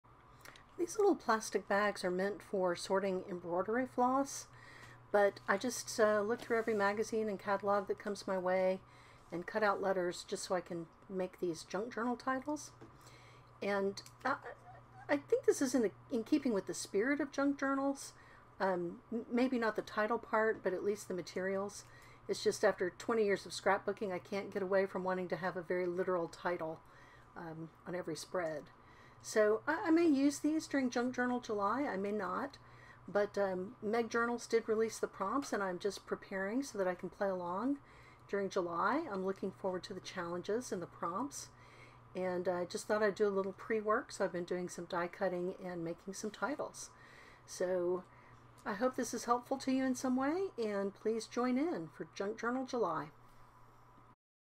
Junk Journal Titles
Getting ready for Junk Journal July challenges with some titles using found materials.  The plastic bags are meant for embroidery floss, so they have a hole in the corner for a binder ring.

Junk Journal July is hosted by megjournals and you can find the prompts